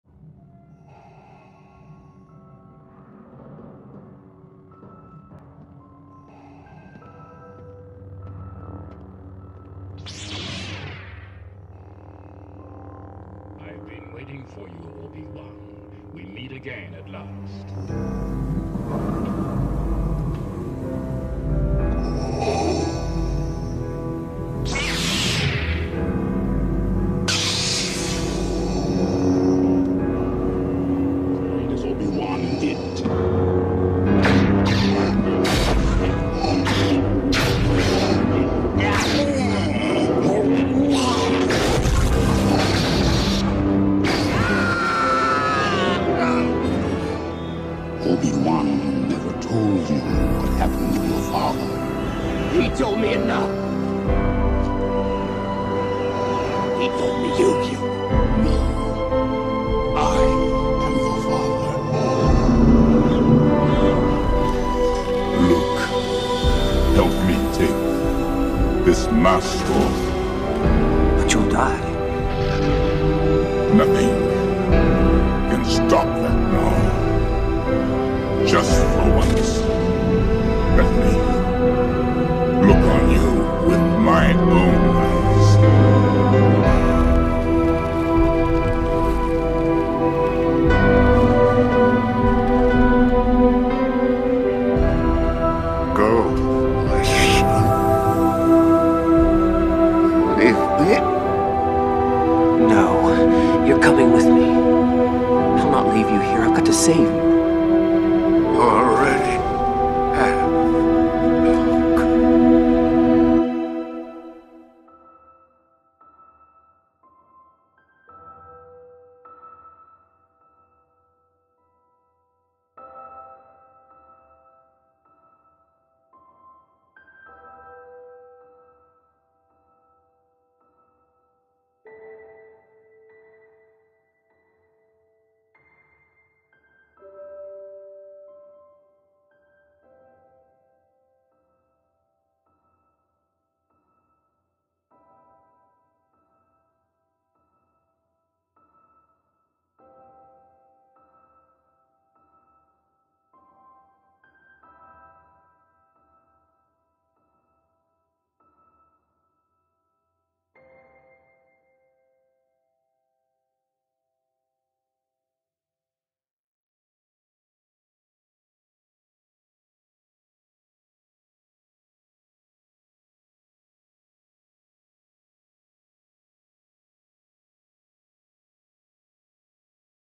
Rest In Peace David Prowse (1935-2020)
Yesterday, David Prowse, British bodybuilder, weight lifter, and actor died due to a severe illness. He is best known for playing Darth Vader in the Star Wars Original Trilogy.
For those curious, James Earl Jones gave Vader his voice, while David Prowse wore the suit to play the character. His impact on Star Wars will be forever remembered, and he shall rest easy in the heavens..
Amen.
Here’s the background music I used.